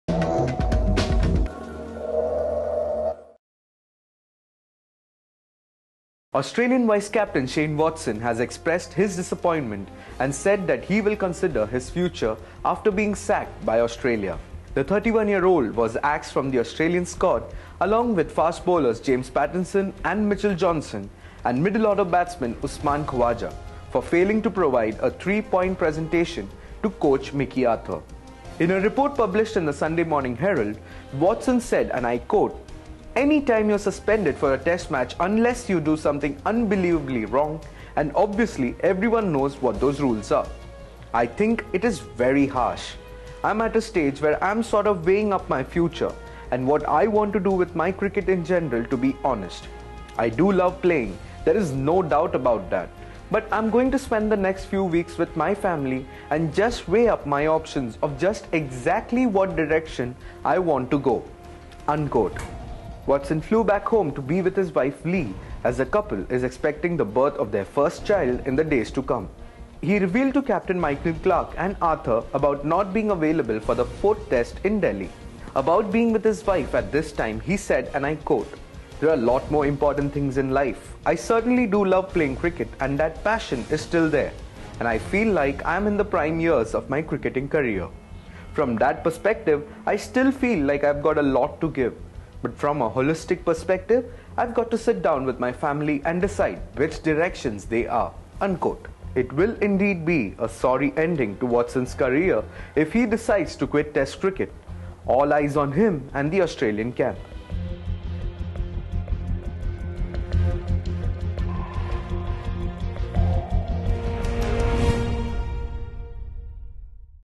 Shane Watson claims axing was 'harsh', to contemplate future
Australian vice-captain Shane Watson has expressed his disappointment and said that he will be considering his future after being sacked by Australia

The 31-year-old was axed from the Australian squad, along with fast bowlers James Pattinson and Mitchell Johnson and middle-order batsman Usman Khawaja for failing to provide a three-point presentation to coach Mickey Arthur.

Watson flew back home to be with his wife Lee, as the couple is expecting the birth of their first child in days to come. He revealed to captan Michael Clarke and Arthur about not being available for the fourth Test in Delhi.